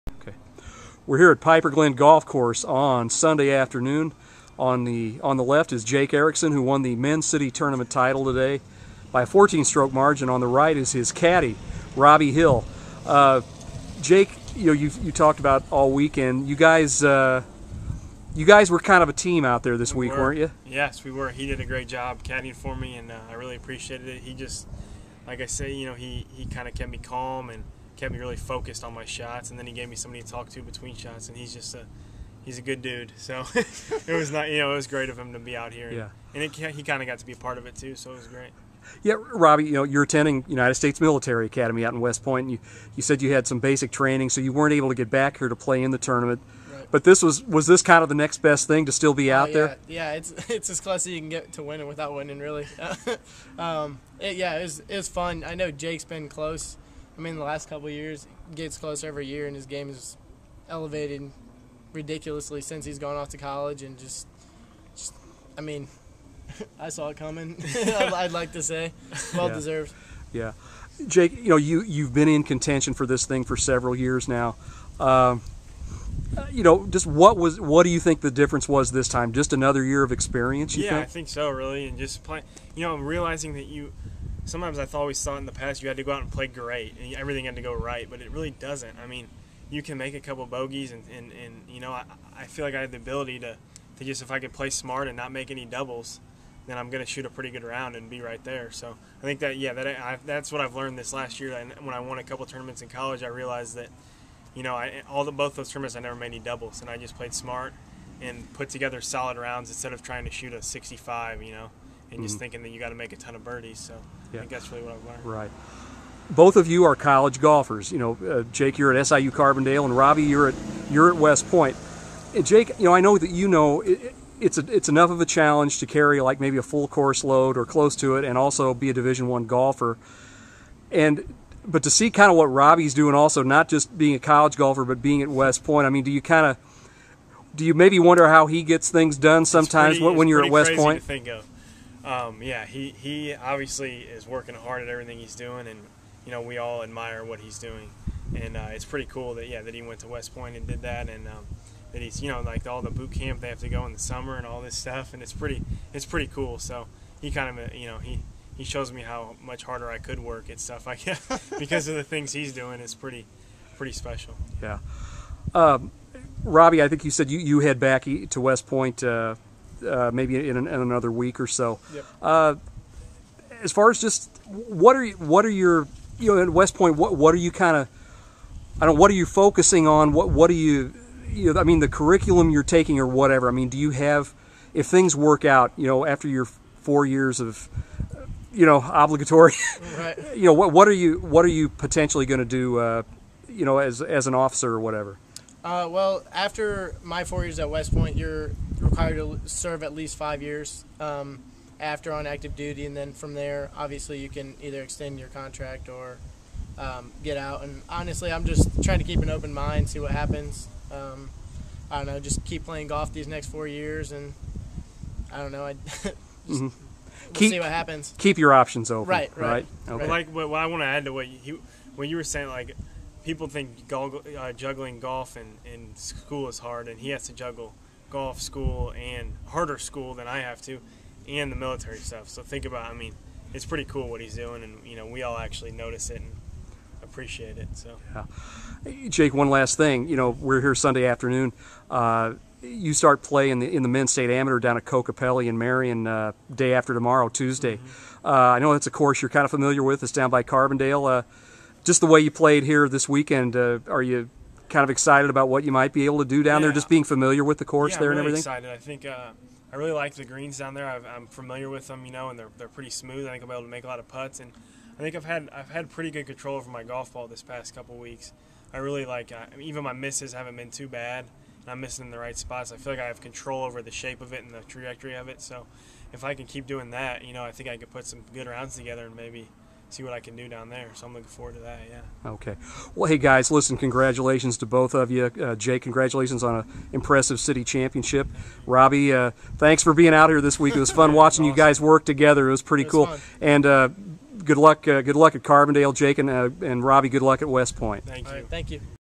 Jake Erickson and Robbie Hill
Jake Erickson, left, won the Springfield Men's City Golf Tournament championship on Sunday, shooting a 73 at Piper Glen for a four-day total of 274 and a 13-stroke victory. Robbie Hill, right, served as Erickson's caddie the final two rounds. Erickson will be a senior at Southern Illinois-Carbondale while Hill will be a sophomore at West Point.